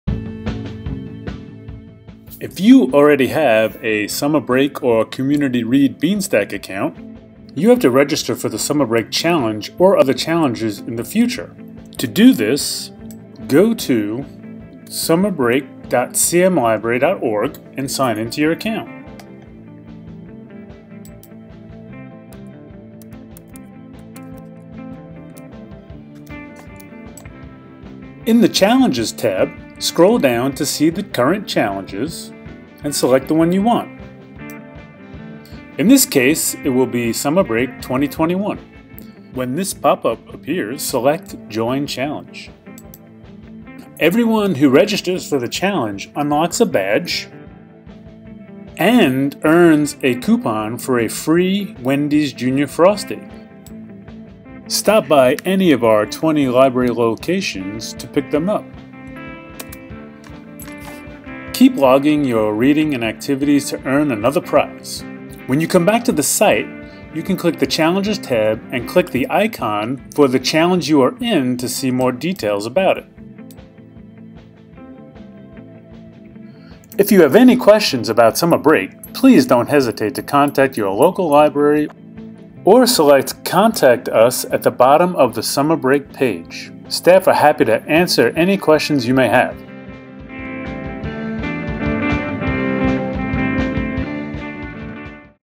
Join a Challenge
Shows how to join a Charlotte Mecklenburg Library Reading Challenge.